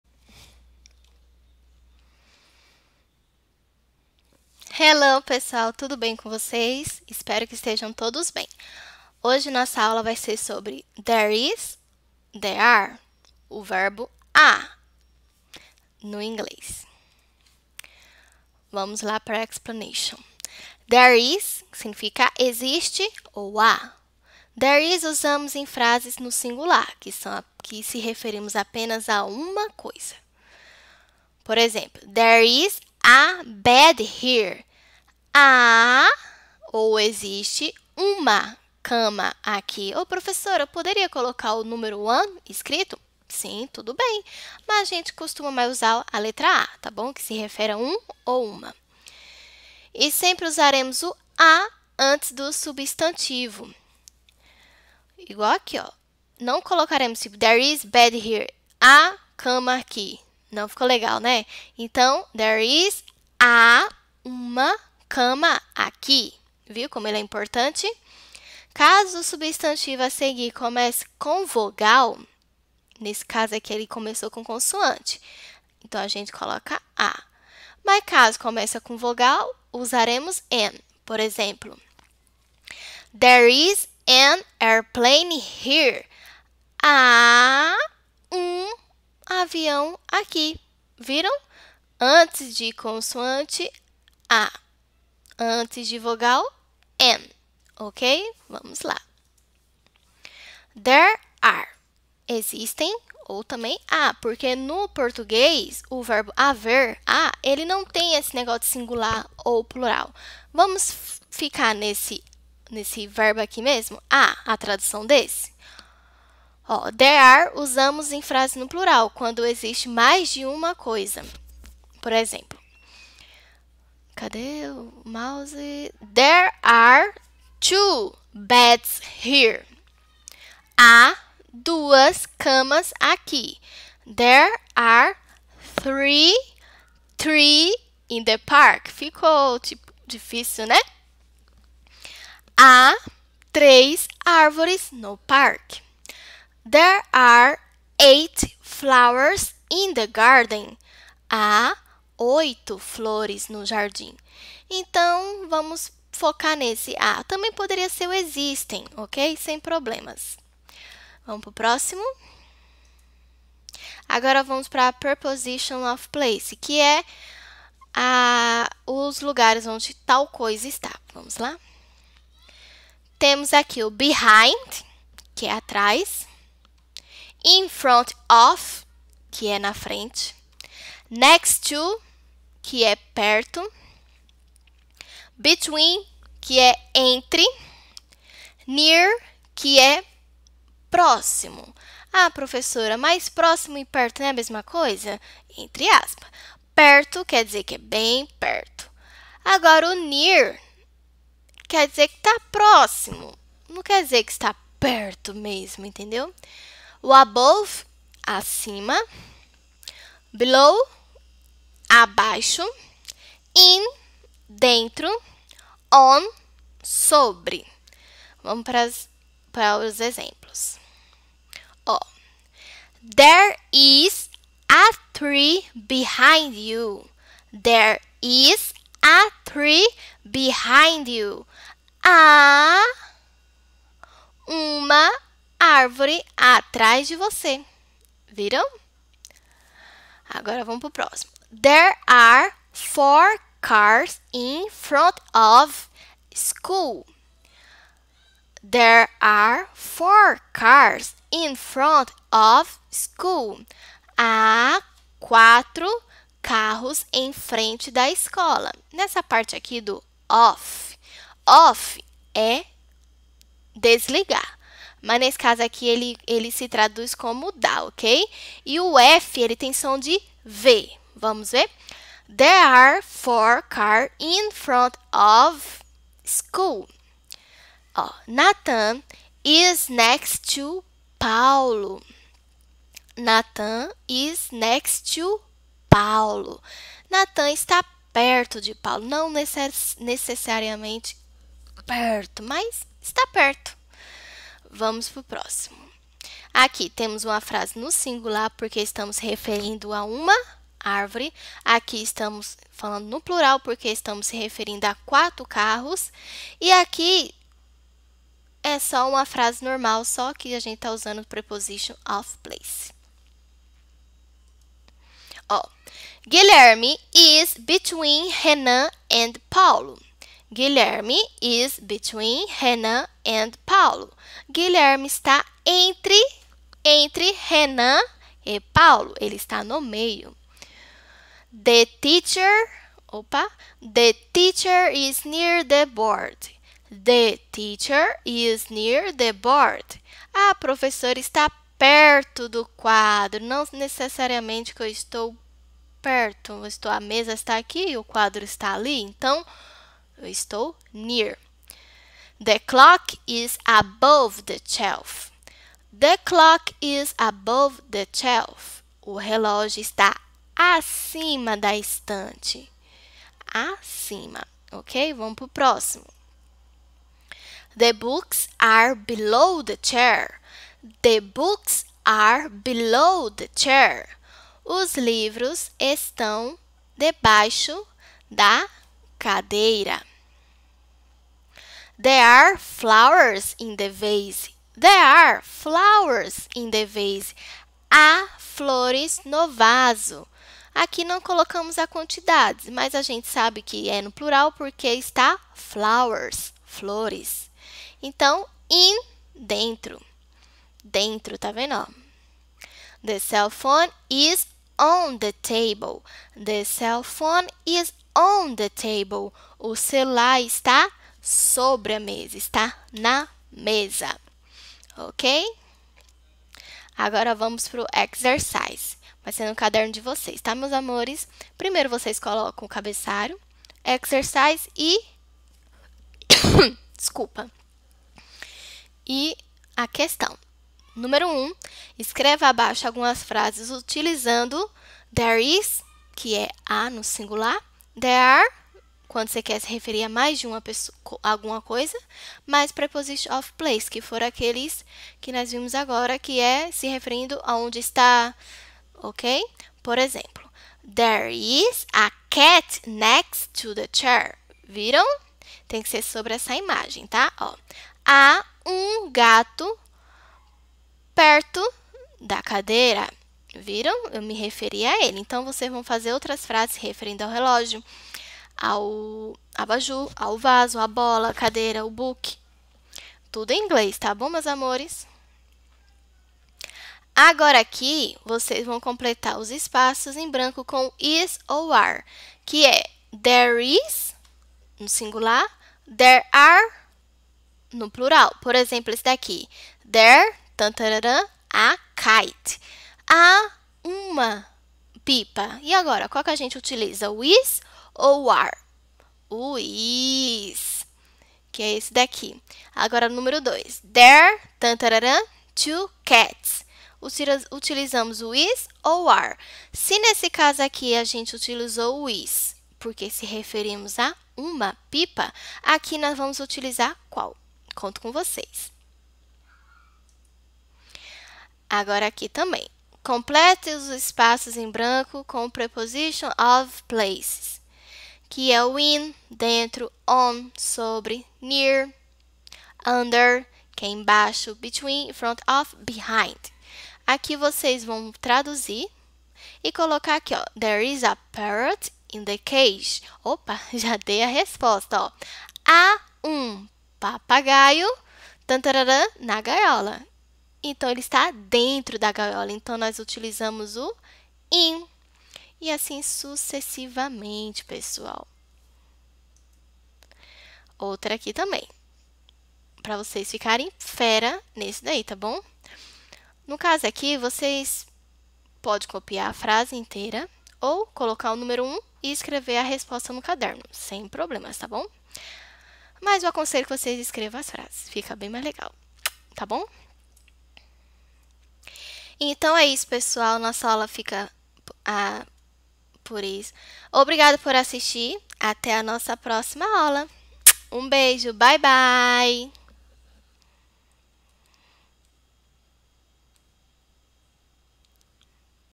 Inglês - Aula do dia 22/05/2020 - Turma: 5º EF
Aula sobre There is, there are e preposition of  place.